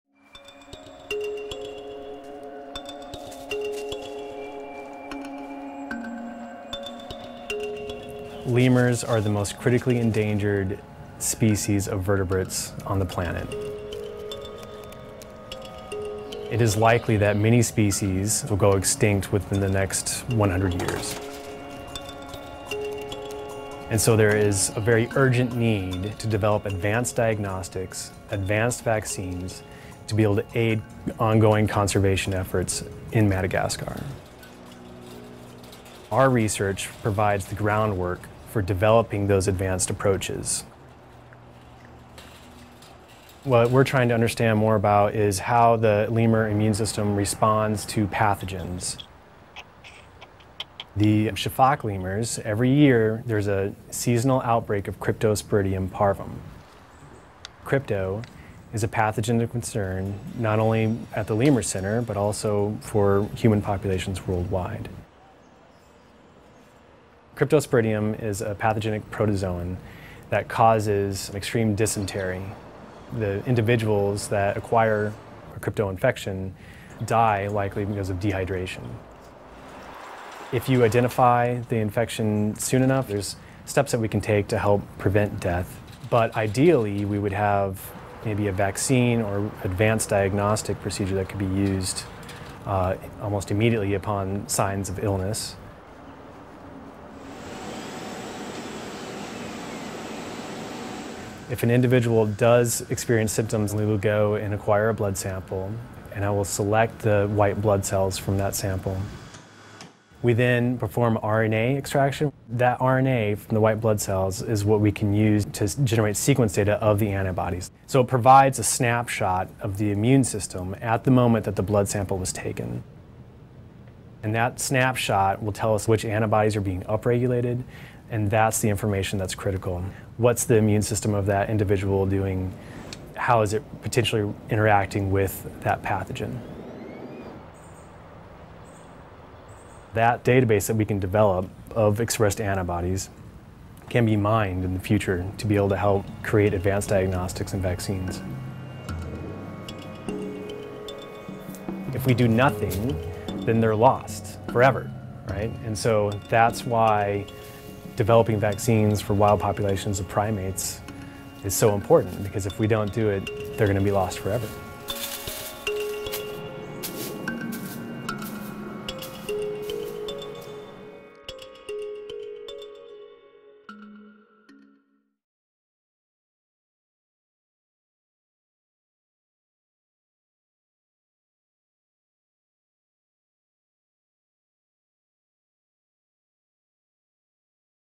疫苗能幫助拯救瀕臨滅絕的狐猴嗎？《國家地理》雜誌
狐猴是地球上最瀕危的脊椎動物，許多種類甚至可能會在未來100年內滅絕。杜克大學（Duke University）狐猴中心的研究人員正在盡一切努力防止這種情況發生。